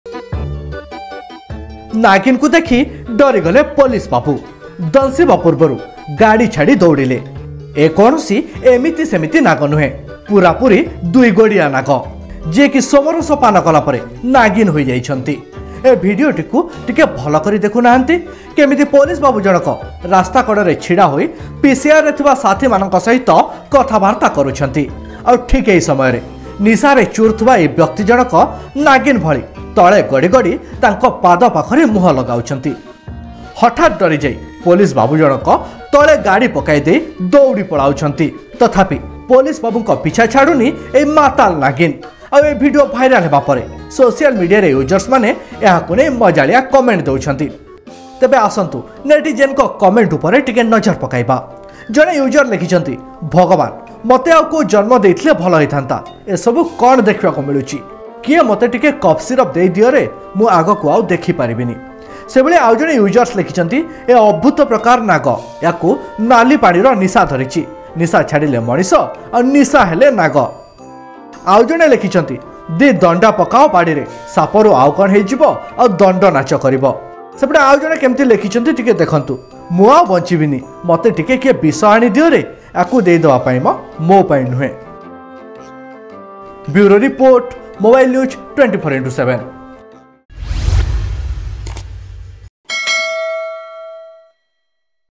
ମାତାଲ ନାଗିନର ଭୟଙ୍କର ନାଚ, ଗାଡ଼ି ଛାଡ଼ି ଘରକୁ ଦୌଡ଼ିଲେ ପୁଲିସ୍ ବାବୁ | Mobile News 24x7
ମାତାଲ ନାଗିନର ଭୟଙ୍କର ନାଚ, ଗାଡ଼ି ଛାଡ଼ି ଘରକୁ ଦୌଡ଼ିଲେ ପୁଲିସ୍ ବାବୁ